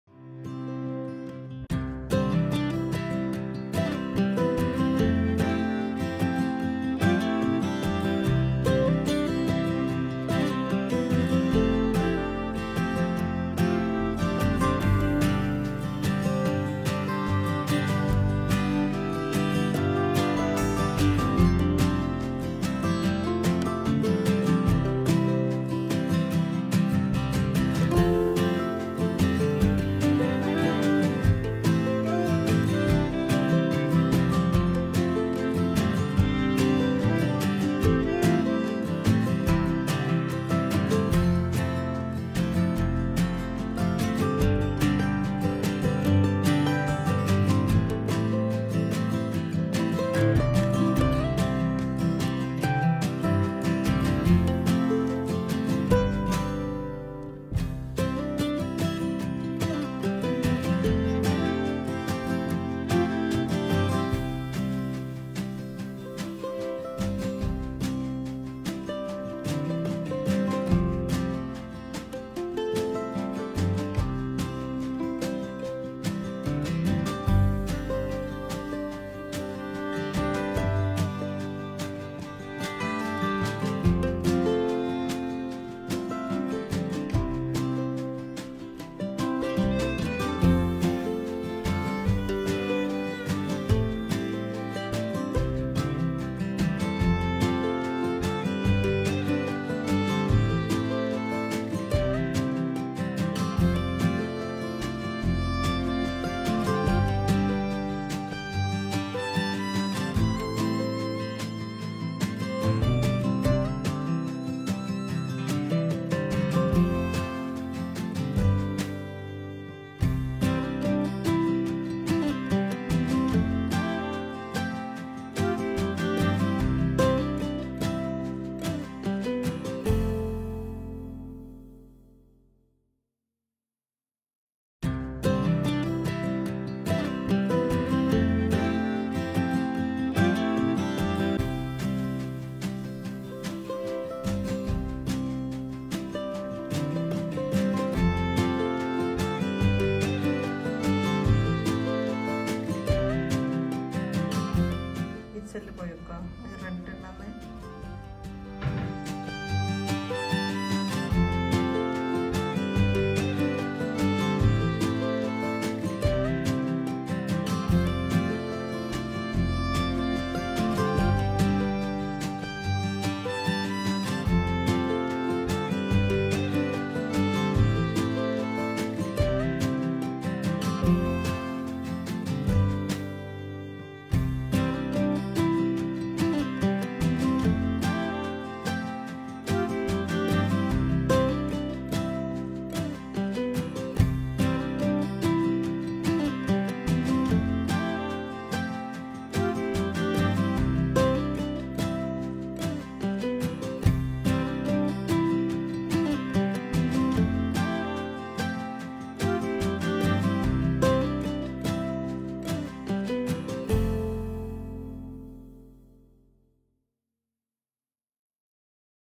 Bottil Art/a simble home decor/Mom Made
Simple bottil arts. Use empty bottil,Acrylic colour and rope.

I have started this channel for sharing recipes and cooking ideas.you can also find fashion and home decor DIYs.